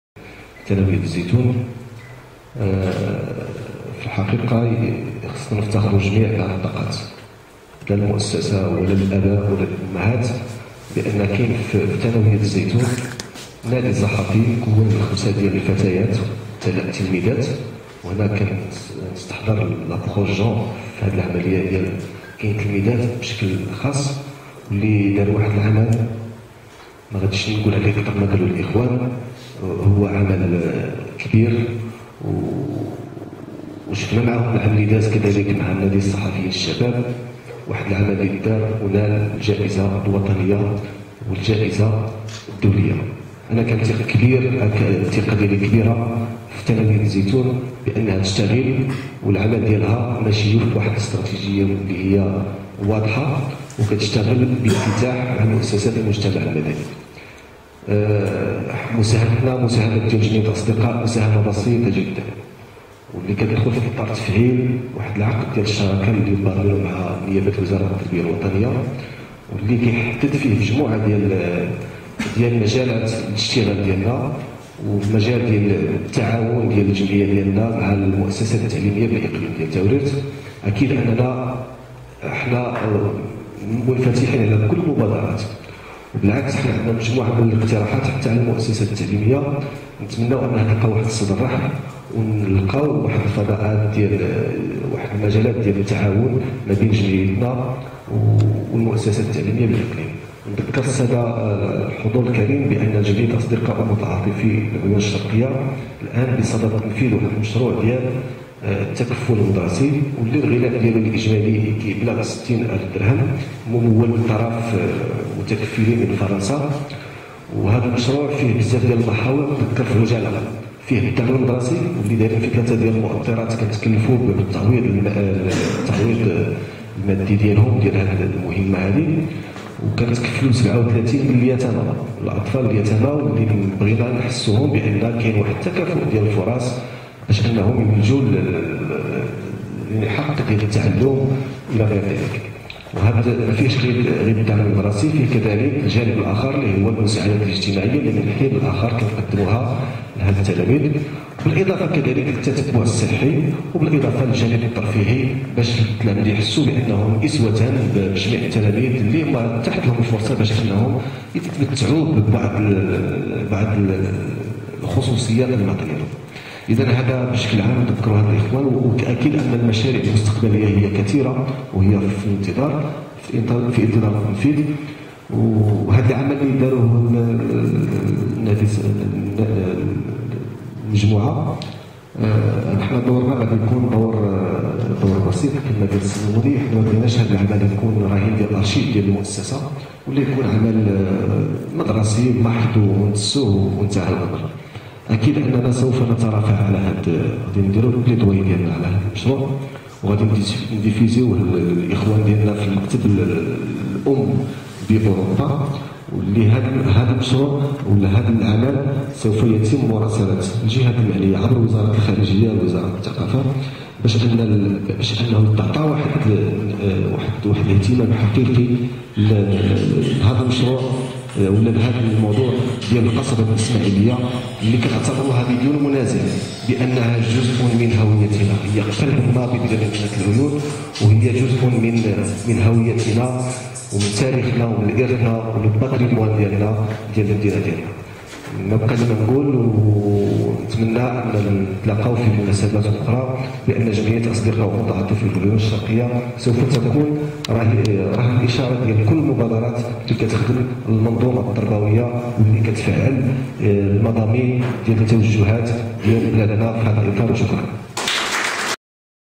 كلمة رئيس فرع ASEO في حق نادي المواطنة
ربورطاج القصبة لنادي المواطنة بثانوية الزيتون التأهيلية

العيون بريس : قامت مجموعة من التلميذات من ثانوية الزيتون التأهيلية بمدينة العيون الشرقية يوم الأربعاء 27 فبراير 2013  بتقديم عرض حول موضوع القصبة الاسماعيلية المتواجدة بالمدينة، و يندرج هذا العمل ضمن الأنشطة التي يقوم بها نادي المواطنة بالمؤسسة المذكورة.

هذا العرض المتميز تم تقديمه باللغة الفرنسية داخل القاعة متعددة الوسائط بحضور عدد من التلاميذ و بعض الأساتذة و مسؤولين محليين و منتخبين اضافة الى فعاليات من المجتمع المدني.

 و قد قدم العرض شروحات مستفيضة حول تاريخ القصبة، و قدم بطاقة تقنية حول الأبعاد و طول الأسوار و عدد الأبراج و الأبواب و المساحة الاجمالية ... ثم تطرق العرض بعد ذلك الى الوضعية الحالية التي تعيشها قصبة العيون الشرقية التي طالها الاهمال و النسيان.

التلميذات اللواتي اشتغلن حول هذا الموضوع كلهن من قسم الثانية علوم فيزيائية 1 ، و يتعلق الامر بالتلميذات :
ازلاف امينة -- الباي ايمان -- كيدون اسماء -- عزيزي لمياء -- حمامي بشرى

و لقد أرسلت التلميذات في ختام عرضهن نداءا الى جميع المسؤولين والى جميع مؤسسات الدولة على المستوى المحلي و الجهوي و الوطني من أجل التدخل العاجل لإنقاذ القصبة الاسماعيلية من الضياع و الانهيار. هذا العرض كان معززا بالصوت والصورة  و كذا شهادات كل من السلطات المحلية والمنتخبين والاختصاصيين إضافة إلى شهادات المجتمع المدني وساكنة المدينة.

العمل الذي تم تقديمه للعموم هو عبارة عن ربورطاج صحفي تم نشره بالعدد الأول لجريدة المؤسسة و التي تحمل عنوان "L'olivier ". وللإشارة فان هذا العمل حظي بإعجاب و تقدير جمعية أصدقاء و متعاطفي العيون الشرقية "ASEO" التي قررت التكفل بنفقات نشر الجريدة و ذلك تشجيعا لهؤلاء التلميذات والتلاميذ.